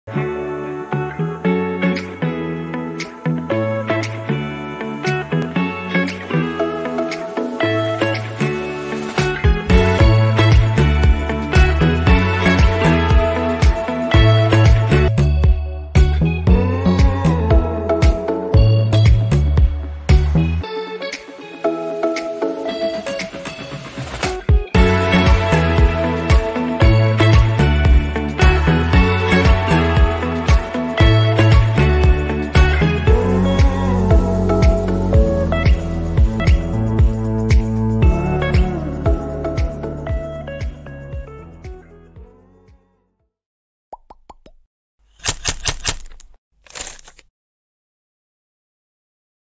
Brewing industry heating equipment-electric heating beer fermentation equipment
This video introduces the 1200-liter beer fermentation tank equipment in the factory